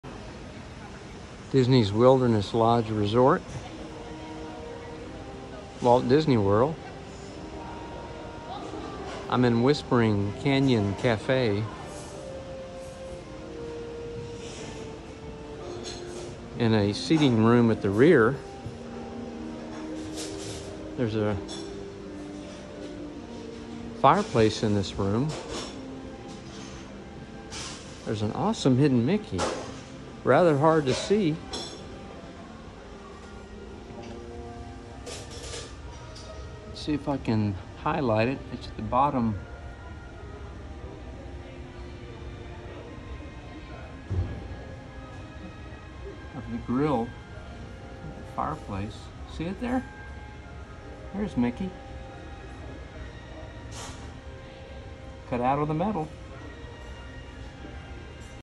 Disney's Wilderness Lodge - Whispering Canyon Cafe Fireplace Hidden Mickey
At a fireplace in the rear room of the Whispering Canyon Cafe, bend down low and look for a classic Hidden Mickey cut out of the outer grillwork.
It's the third icon from the left corner along the bottom row of icons.
Disney's Wilderness Lodge Whispering Canyon Cafe